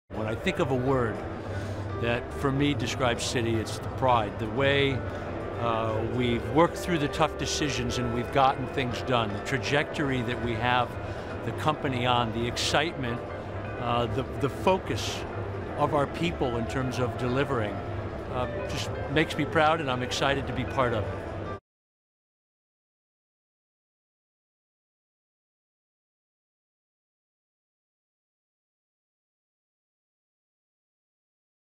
Citi CEO Michael Corbat at Citi’s 2017 Investor Day, Part 2
Citi CEO Michael Corbat shares the one word that best describes Citi for him at Citi’s 2017 Investor Day.